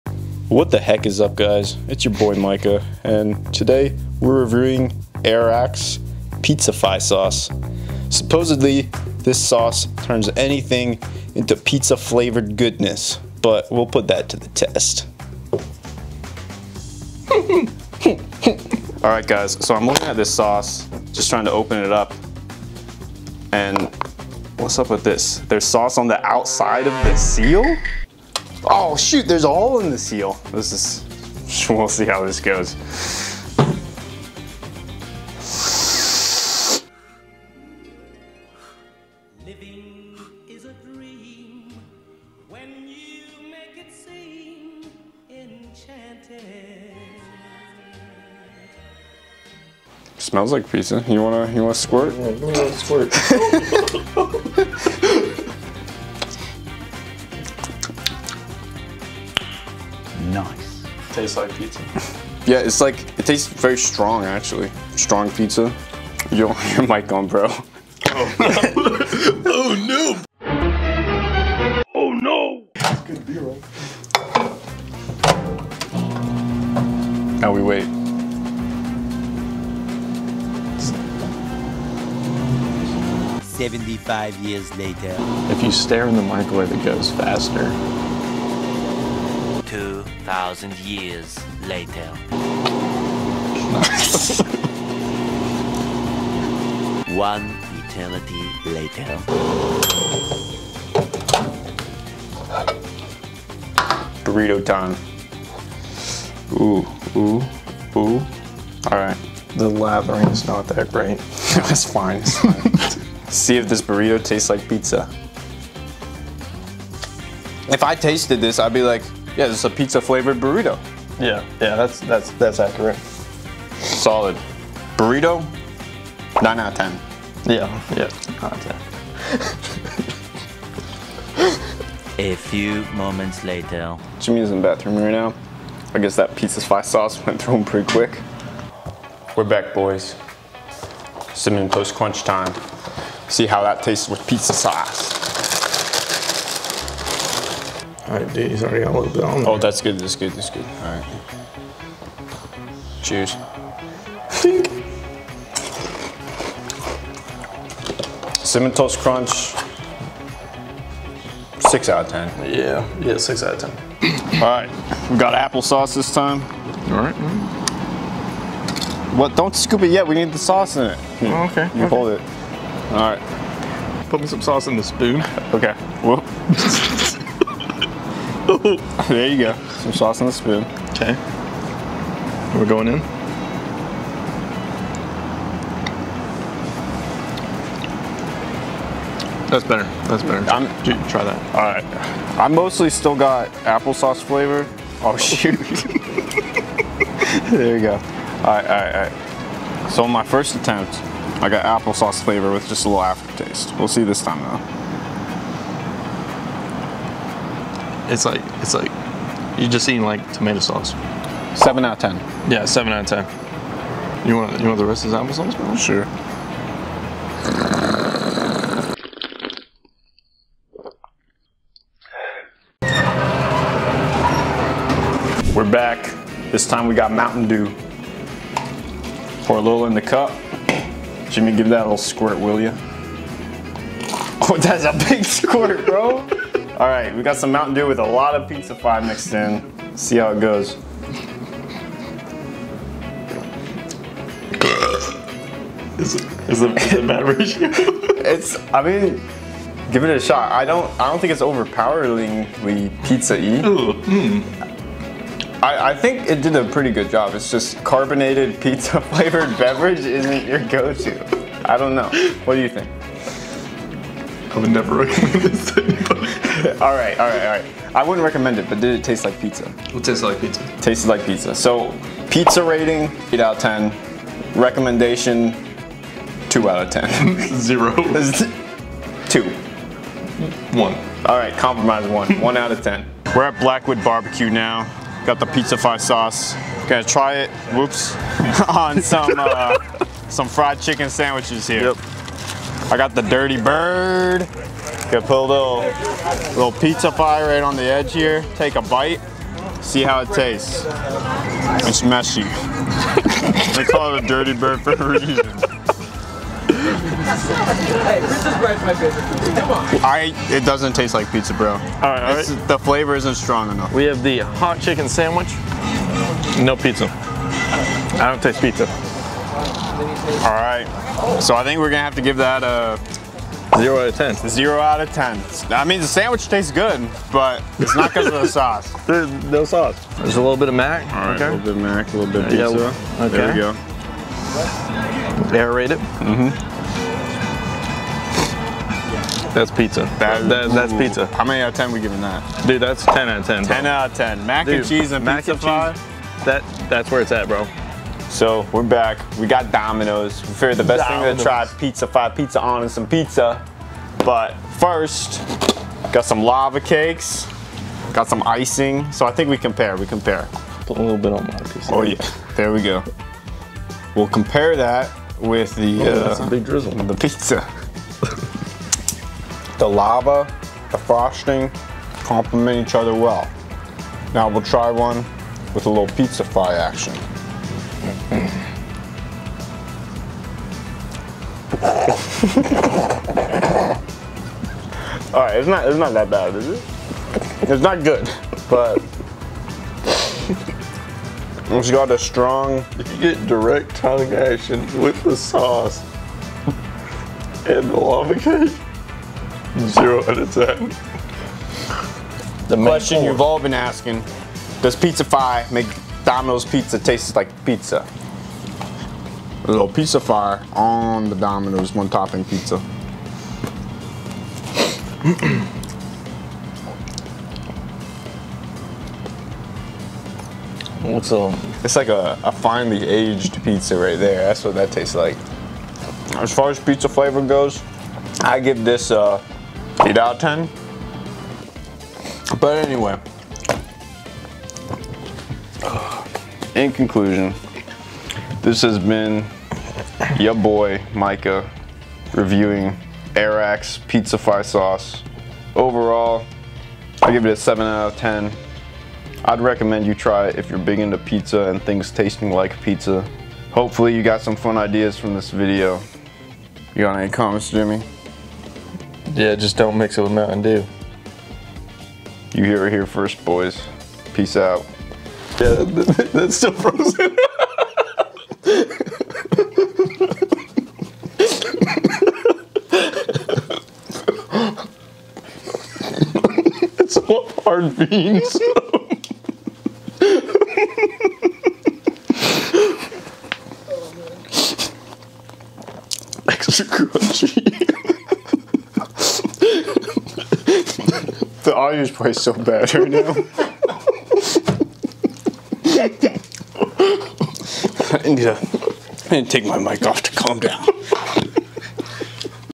Apple Sauce Tastes Like Marinara?! (Pizzafy Review!)
The boys and I try @airrack's famed Pizzafy Sauce!
Shout out to @eli.willsea for filming the video, and shout out to Jimmy for being my co-host